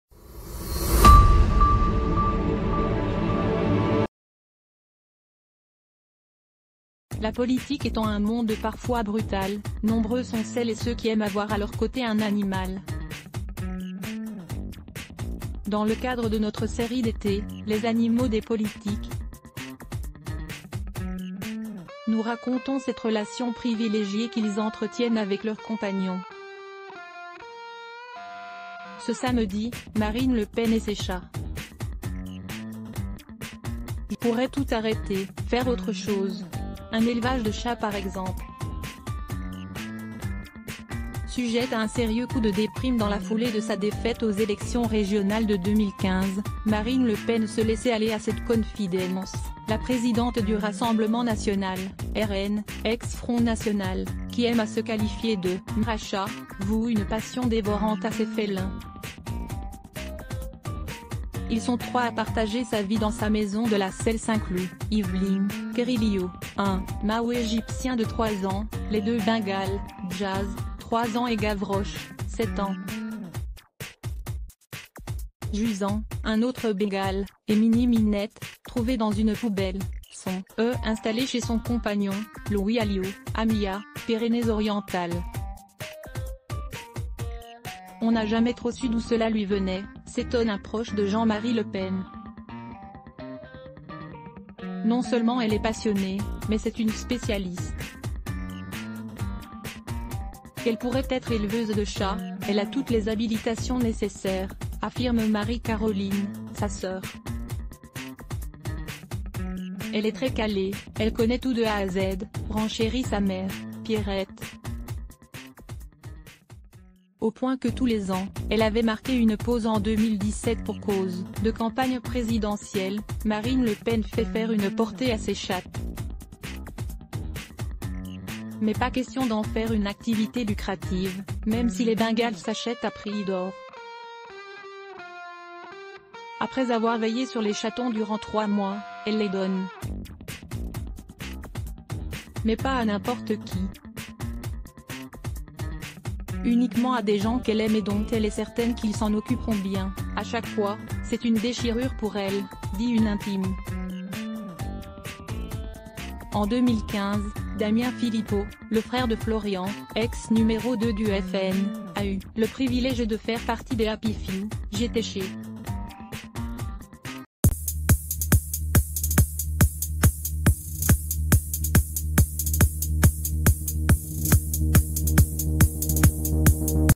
Les animaux des politiques : Marine Le Pen, la «mère à chats»
«Non seulement elle est passionnée, mais c’est une spécialiste. Elle pourrait être éleveuse de chats, elle a toutes les habilitations nécessaires», dit de Marine Le Pen sa sœur Marie-Caroline